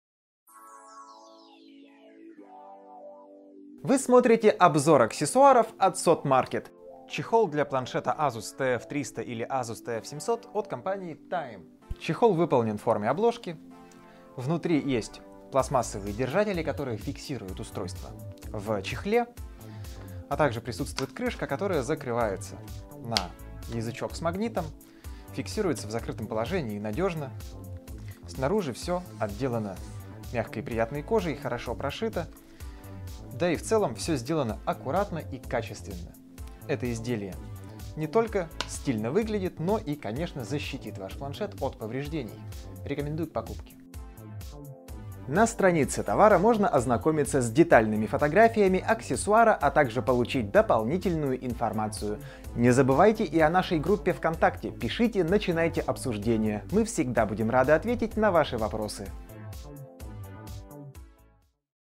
Видео обзор Time для Asus TF300T от Сотмаркета
Купить чехол Time и узнать дополнительную информацию можно на сайте магазина

Time — чехол совместимый с Asus TF300T. Надежно защитит планшет от царапин, повреждений, влаги и пыли. Помимо этого чехол будет служить стильным и оригинальным аксессуаром.